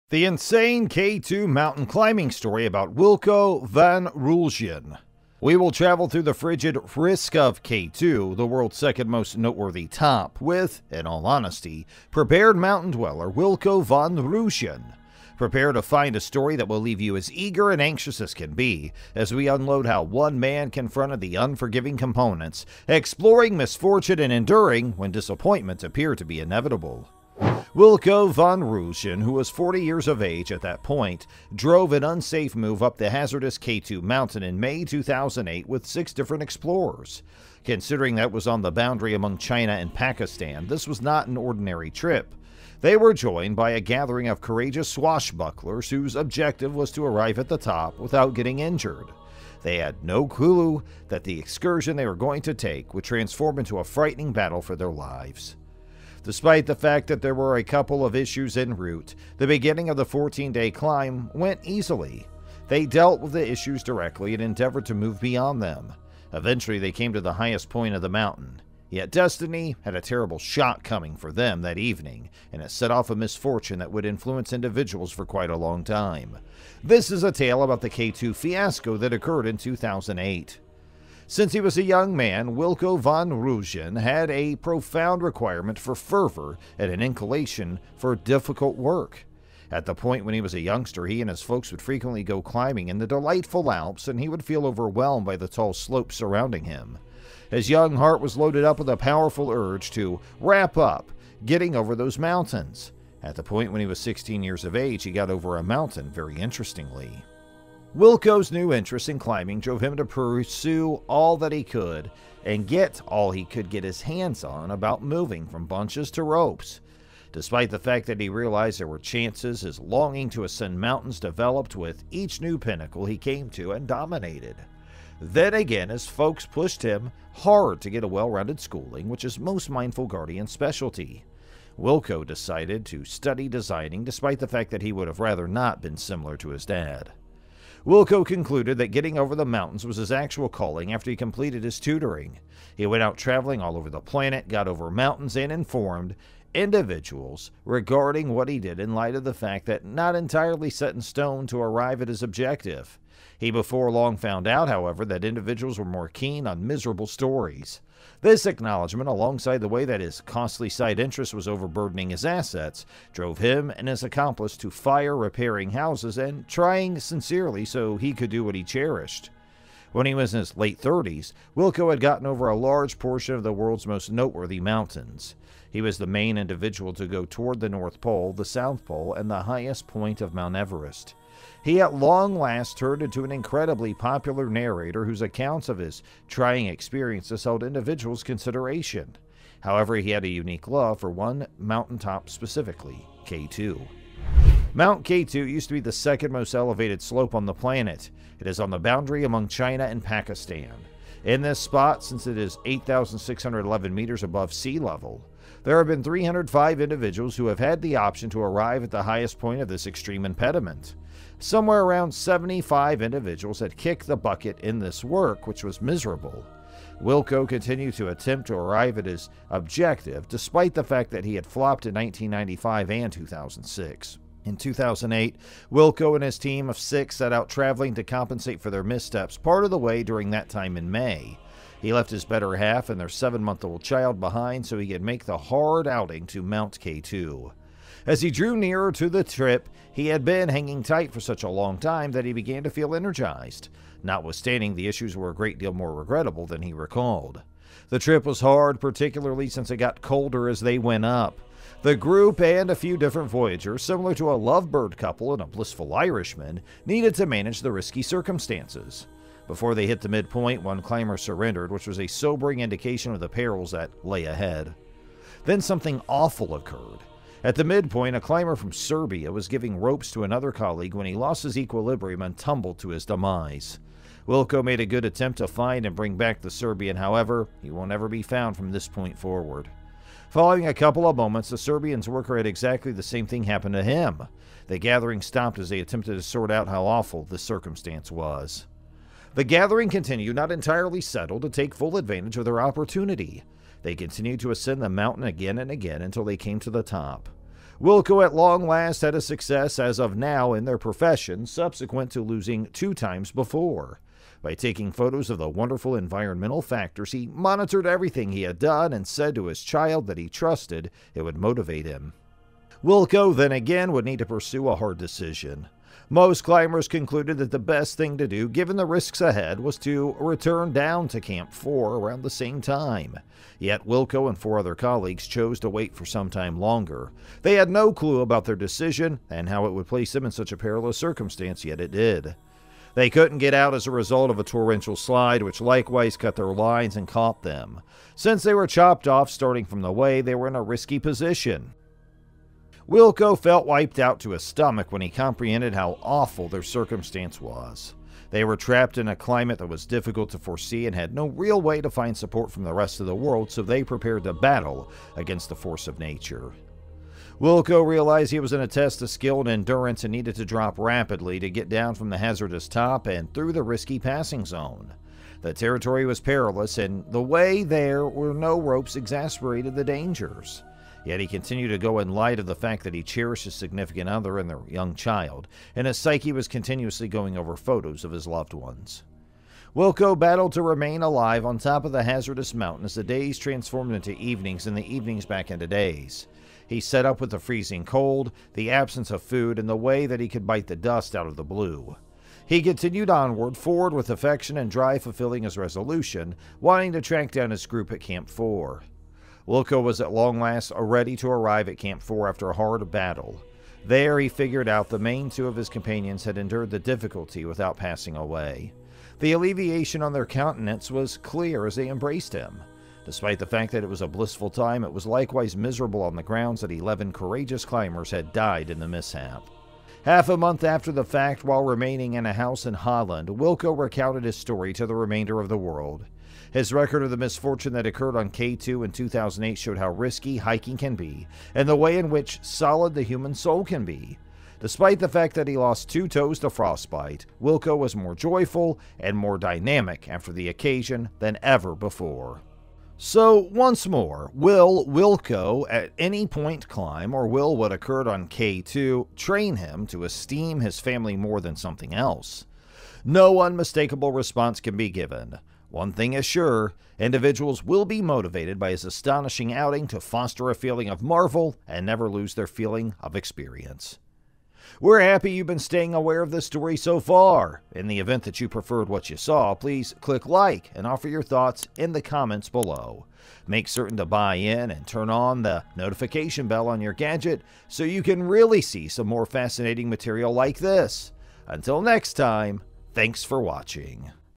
The INSANE K2 Mountain Climbing Story About Wilco Van Rooijen
Prepare to be immersed in a spine-tingling narrative that unveils the harrowing events that unfolded on the treacherous slopes of K2, the world's second-highest mountain. Brace yourself for a gripping tale that will leave you breathless, as we delve into the shadowy side of mountain climbing and witness a disaster that shocked the world.